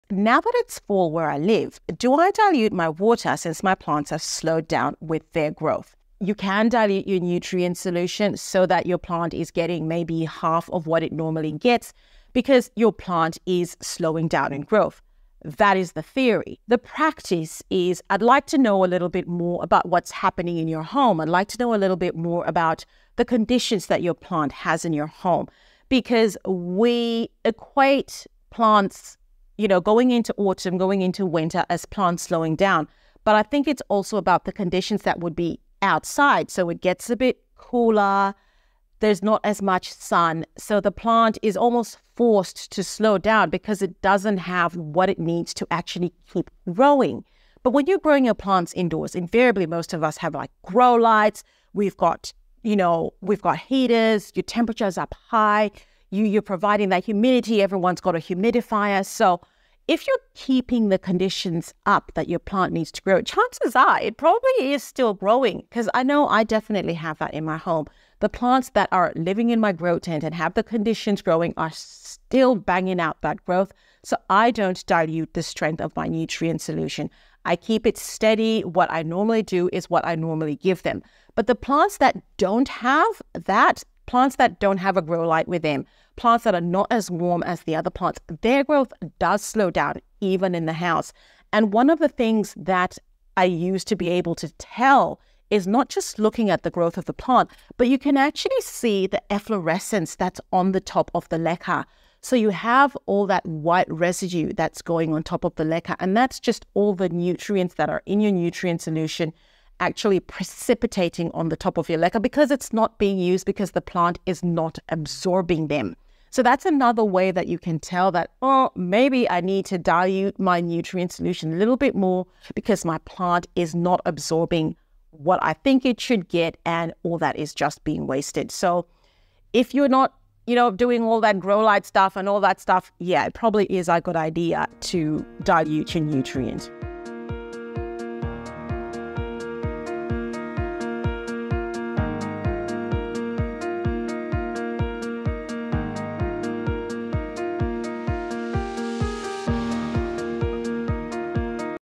Do I Really Need to Dilute Plant Nutrients in Winter?
Should you dilute your nutrient solution in winter?
*Nutrients*
*Grow lights*
MotherLife Plant Spectrum
Soltech Grow Lights
Barrina Grow Lights
*Light Meter*
*Pots*
*Plant accessories*
*Products I use or recommend to care for plants*
👉 Amazon Store